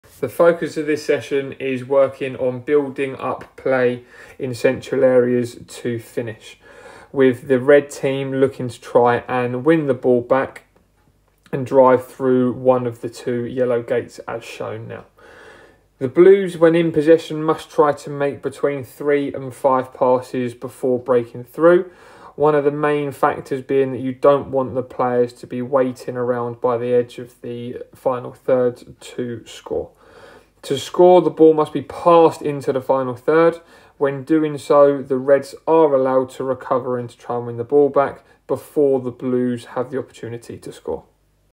Working on building up play in the central areas to then finish
Here is a fantastic session to use with your Grassroots football teams to help players with working on building up play in the central areas to then finish.

Did you know we send out session plans for various age groups each week to save you planning them?